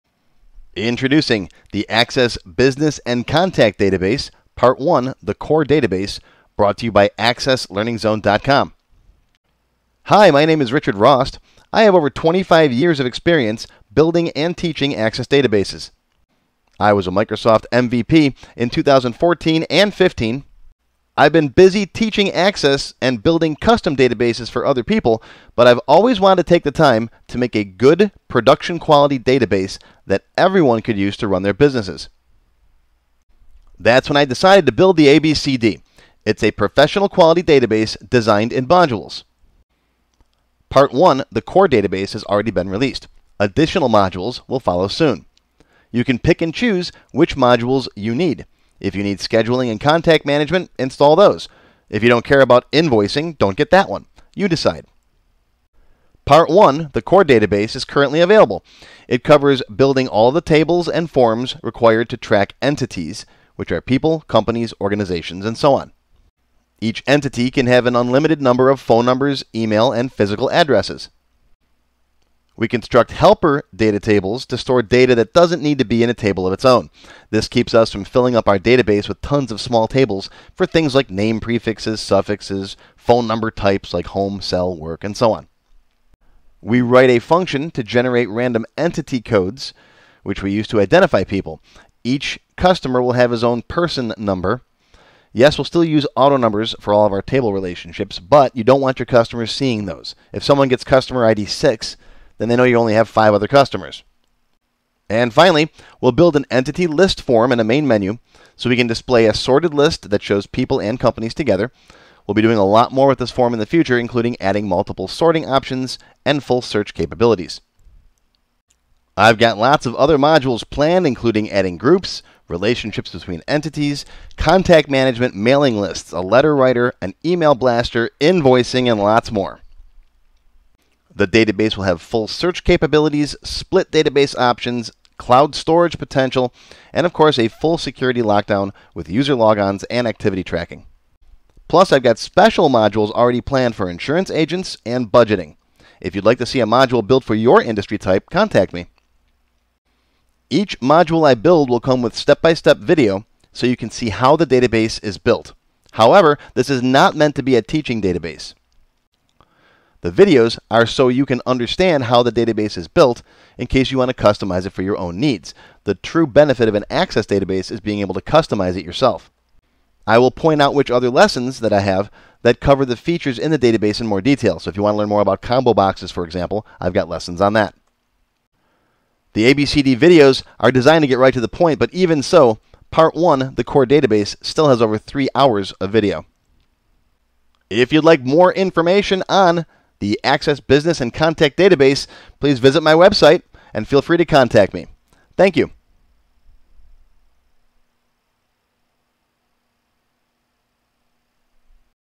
ABCD Access Business & Contact Database Part 1: Core is Now Available
The Access Business & Contact Database (ABCD) is designed to be a professional, full production-quality database to cover all of your information management needs. I have been building and teaching Access databases for over 25 years. Now, I'm finally building a solid, professional database system that you can use to run your business.

I will record videos showing how the database was built, but mostly so that you can understand the construction and be better equipped to make any modifications or additions you may need. This will be marketed as a finished database without the need for any enhancements, but, of course, one of the main reasons to use Access is so that you can easily make changes. The videos will be a quick run-through of how each feature is built, as I'm building it. I will be using advanced features like SQL and VBA from the start, so you may want some SQL or  VBA Developer Lessons under your belt if you're going to try to follow along.

ABCD Part 1: Core Database

The first release is the ABCD Core Database, which is now available along with over three hours of video. This is the "heart" of the database and will include tables, forms, and everything else needed to track people and organizations/companies called Entities, which are at the heart of the database. Unlike the simple databases I've built in my classes, each person will be able to have multiple unlimited phone numbers, addresses, and email accounts associated with them. And of course, these will be properly normalized one-to-many relationships.

Something new I'll be building in the ABCD Core that I've never done in any of my classes is something called a Helper Data table. This will replace having dozens of small tables in your database to store things like name prefix (Mr., Mrs., Miss), suffix (Sr, Jr, III), gender, marital status, relationship type, phone number type (cell, home, work), and so on. Instead, all of this "helper" data will be stored in a single table. This will make our database more organized and keep the overall number of objects in our database to a minimum.

ABCD Add-On Modules

After the Core package is finished, I will be releasing new Modules on a regular basis. These will be designed so that you can pick and choose which features you want and don't want in your database. Install only what you need. Some of these will include:

Contact Management
Groups & Lists
Letter Writer (Bulk Email, Print)
Task Management
Schedule & Calendar
Importing & Tracking Leads
Orders, Quotations, Invoicing
Recurring Memberships
Employee Time Tracking
Inventory & Barcoding

Plus, the ABCD will eventually have full enhanced search capabilities, split database options, possible cloud-based data storage, and full security to lock the database down. I'll be adding these and a lot more features - pretty much everything I've covered in my classes - to the system, eventually.

I'll even be adding custom Modules for specific vertical markets like insurance (tracking clients, leads, policies), automotive (vehicle & fleet maintenance), schools (students, attendance, grades), budgeting, and lots more.

Your Feedback

The ABCD Core Database package is now available. I will be releasing new modules on a regular basis. You can see the list of features that are scheduled for development on the Coming Soon page. I would very much like your feedback on what features you think should be available. If you have a specific interest for your business type, let me know.